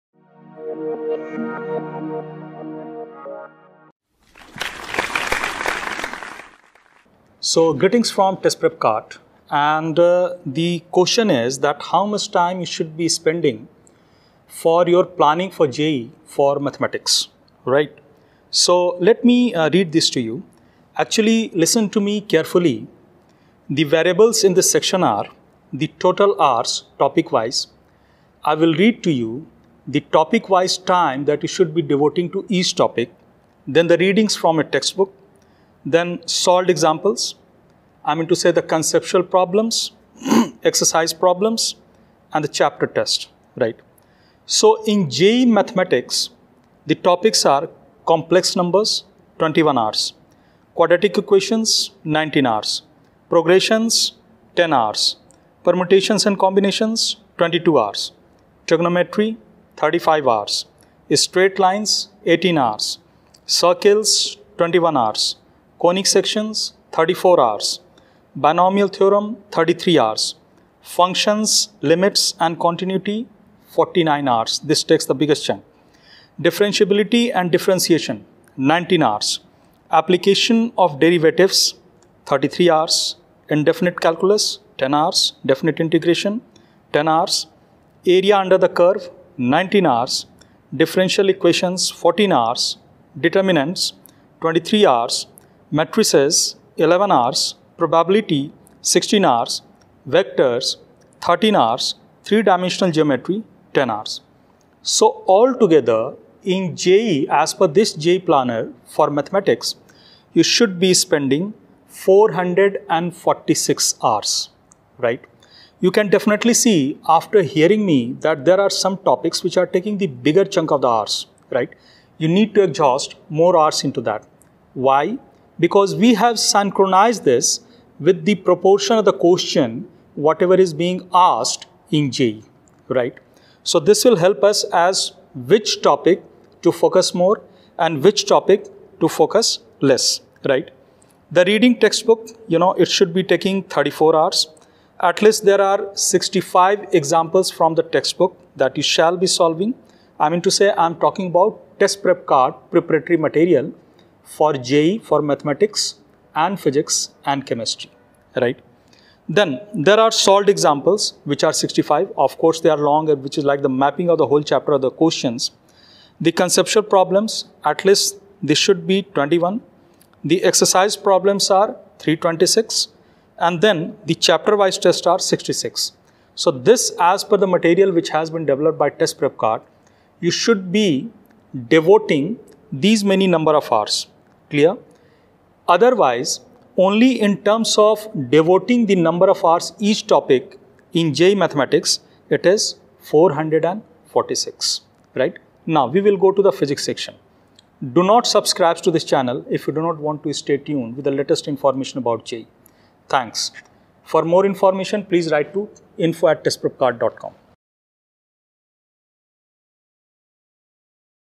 Best JEE Mathematics Study Planner for all JEE Aspirants
In this Video,Our Experience Counselor will let you know the total number of Hours One should Devote to each Mathematics topic while He/She preparing for JEE Mathematics.

To view the complete Physics, Chemistry and Mathematics video for free and to download JEE subject study material, please follow the URL below.